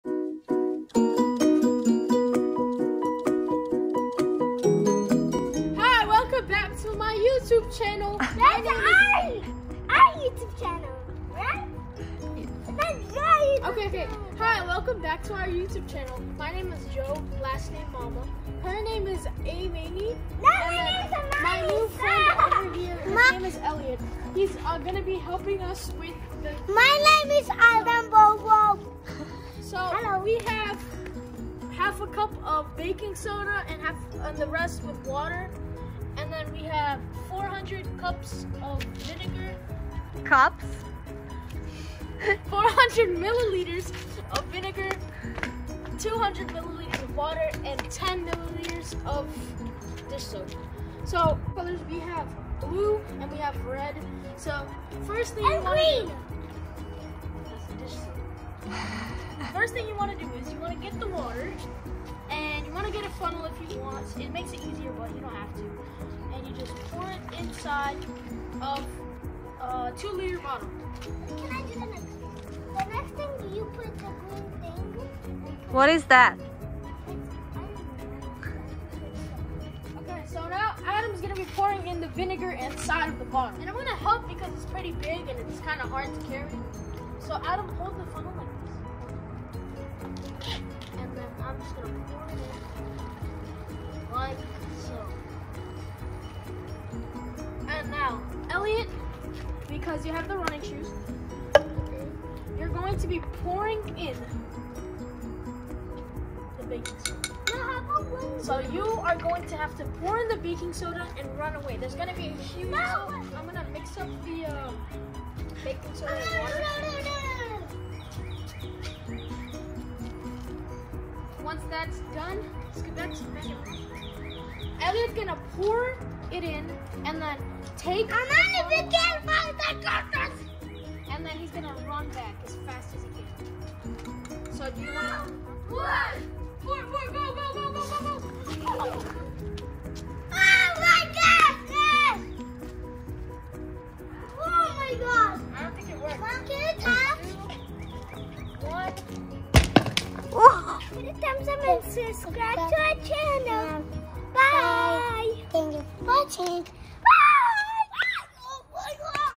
Volcano In The Bottle
Amani and Adam creating a Volcano with uncle AJ and his friend Elliot